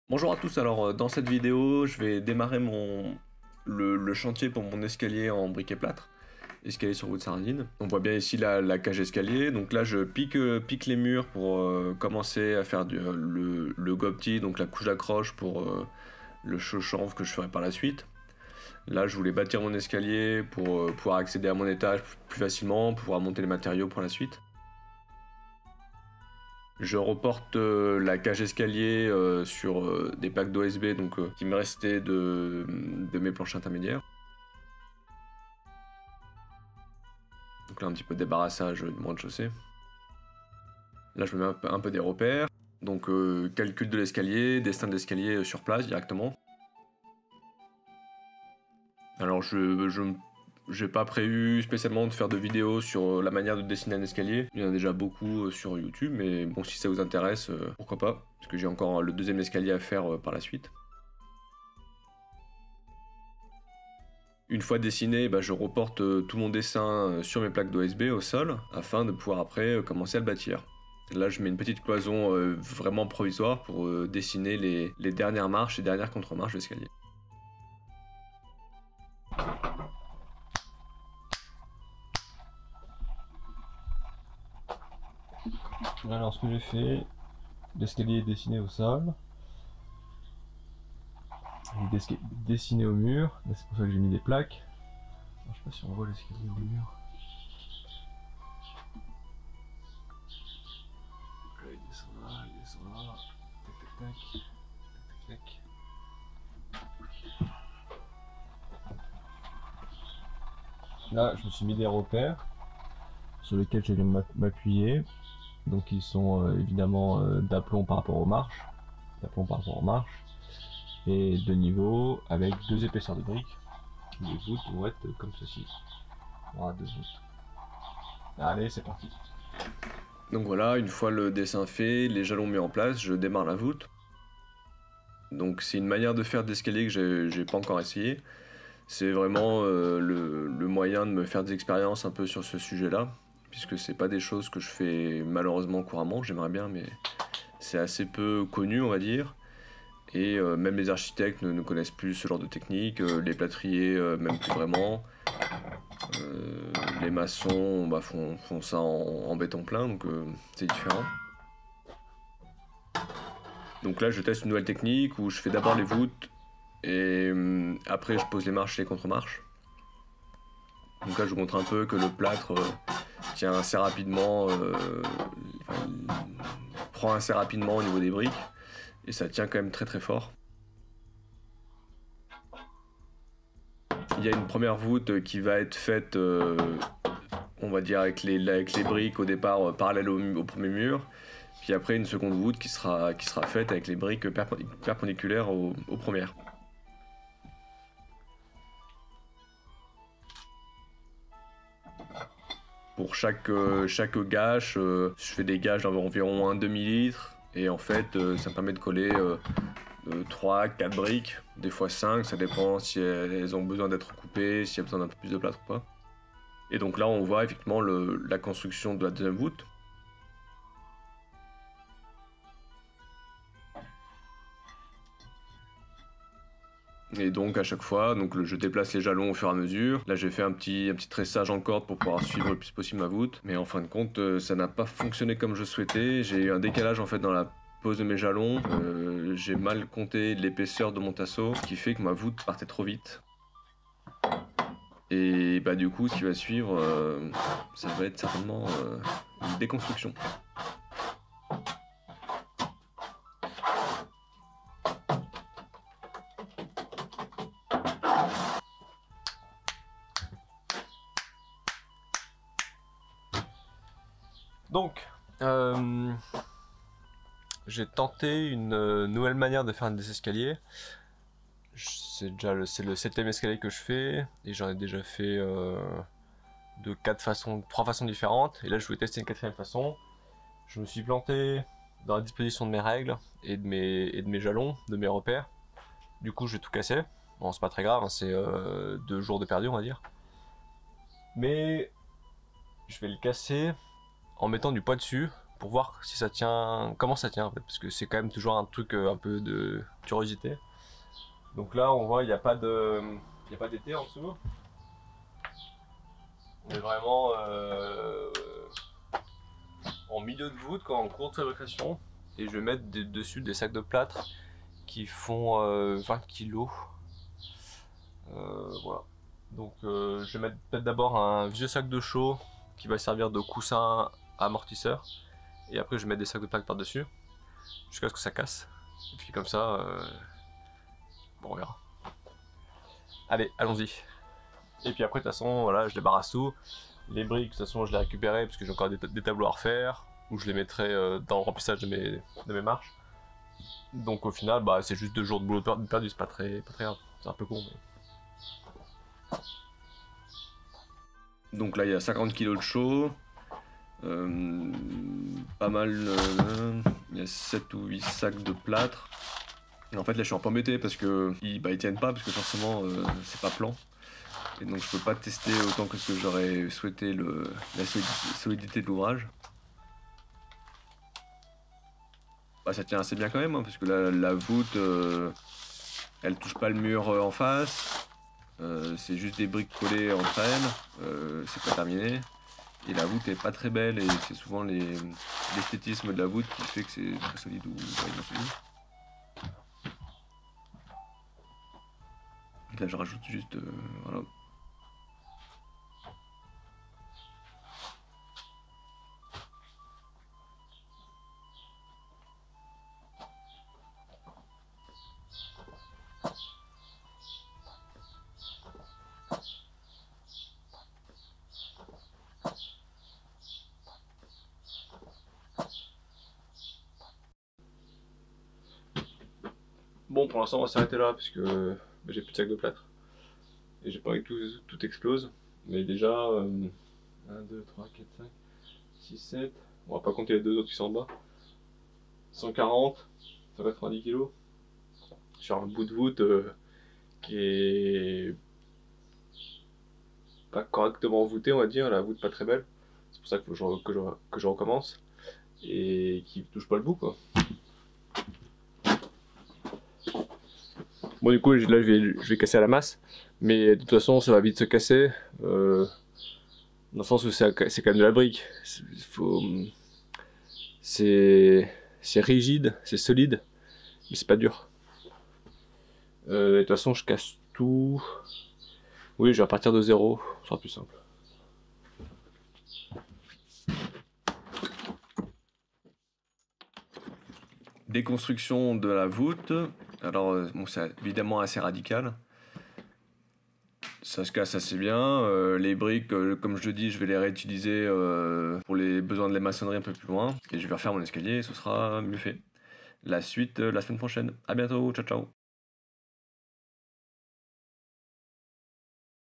Escalier sur voûte sarrasine. Escalier en briques et plâtre [1/4].
Démarrage de mon escalier, tracé et implantation, démarrage des premières voûtes.

La suite au prochain épisode.